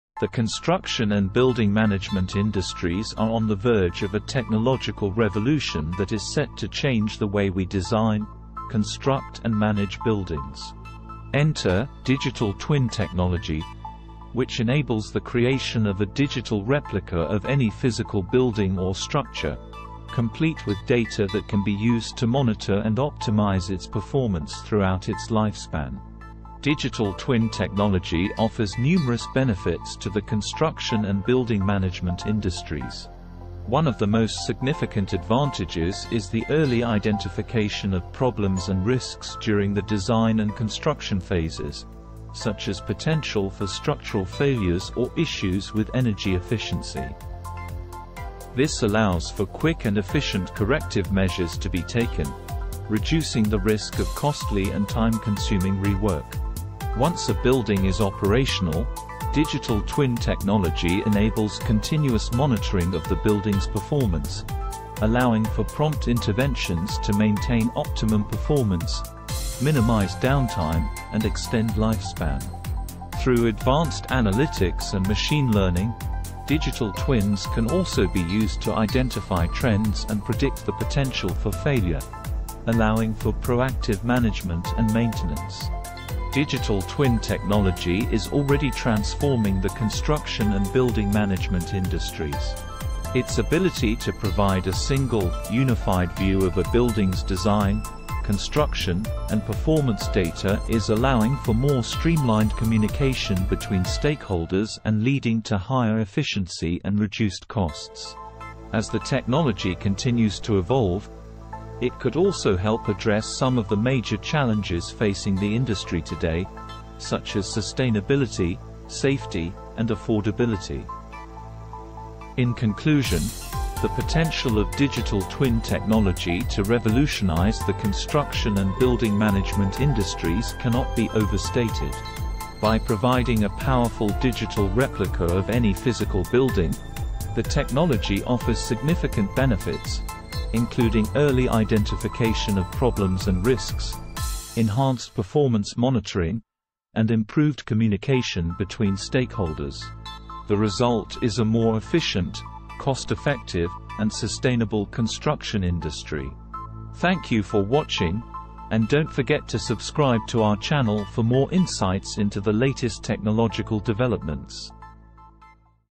The Future of Construction Management: Digital Twin Technology
In this video, we're going to discuss the future of construction management, specifically digital twin technology.

Digital twin technology is an exciting new area of construction management that has the potential to revolutionize the industry. We'll discuss the advantages and disadvantages of digital twin technology, and discuss some of the applications that digital twin technology has already been used in. Finally, we'll give you a sneak peak of our upcoming course on digital twin technology, which will be available in May 2019!
In today's fast-paced and ever-changing world, the construction and building management industries are constantly seeking innovative solutions to improve efficiency and reduce costs. Enter digital twin technology, a cutting-edge tool that is transforming the way these industries operate.

Digital twins are digital replicas of physical entities, such as buildings or projects. They allow for real-time monitoring and simulations, providing invaluable insights into the construction and management process.

In this video, we explore the benefits of digital twin technology for construction and building management industries. We discuss how it can streamline design and construction processes, reduce project timelines, improve safety, and lower costs.

We also examine how digital twin technology can revolutionize facility and asset management, providing better control over energy consumption, maintenance, and overall building performance.

With the power of digital twin technology, the possibilities for the construction and building management industries are endless. Join us in discovering the future of these industries by watching this video.